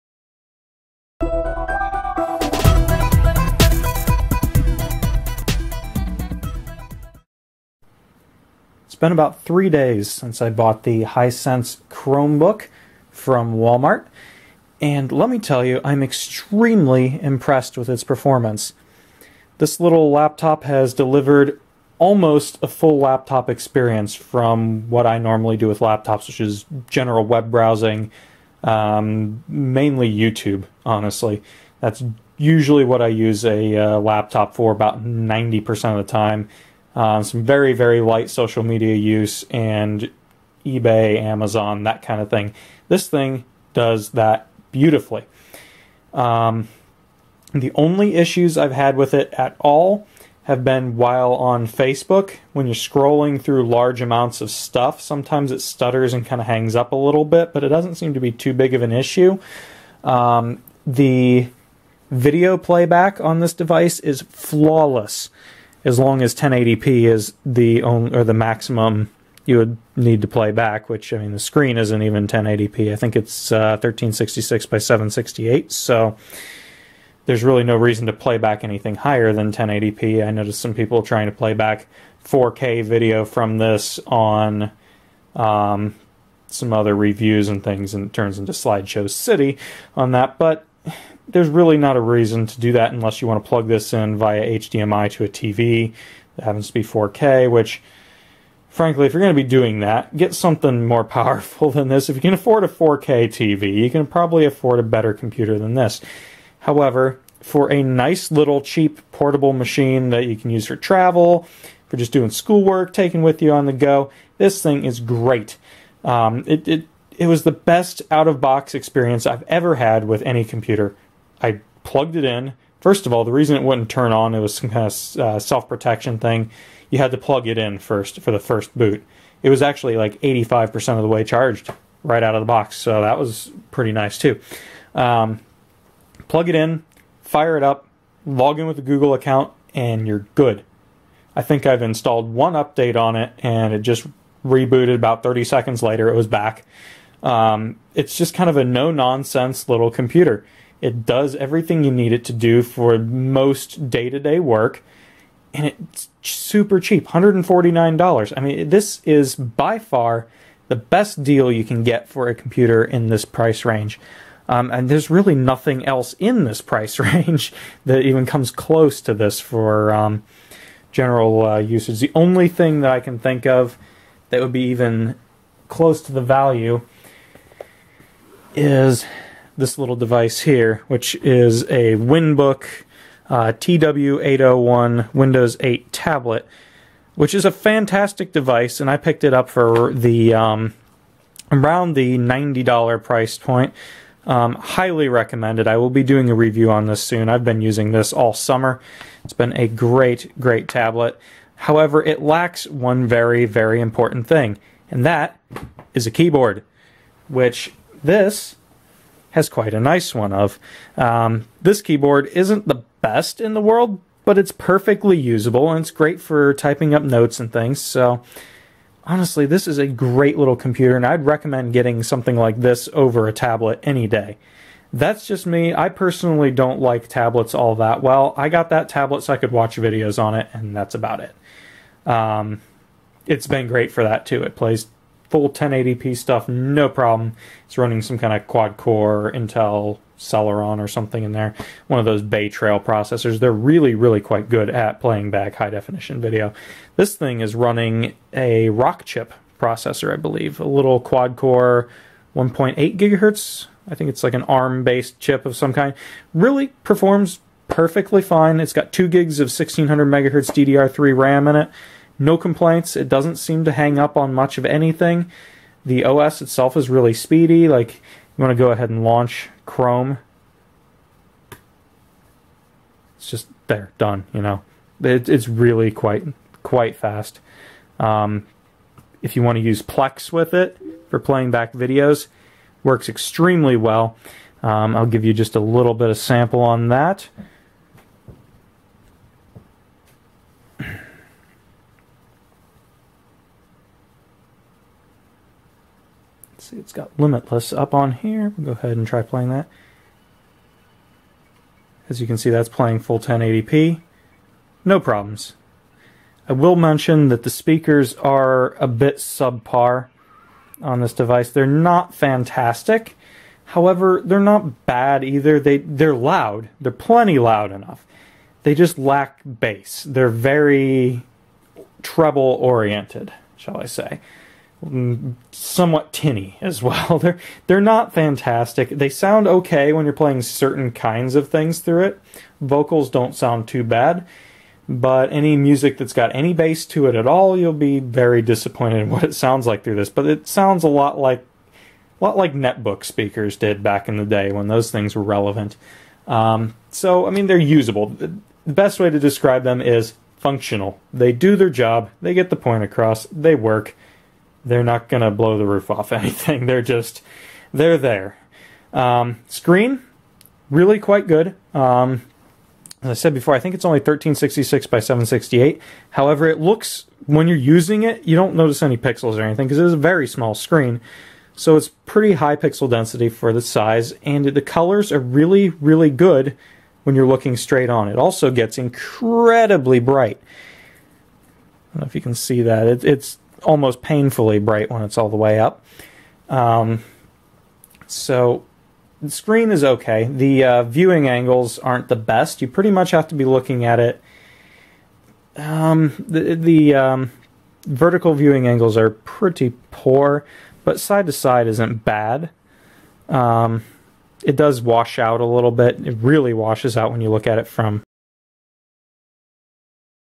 Hisense Chromebook Update
A more in depth look, and overall impression of the Walmart exclusive Hisense Chromebook.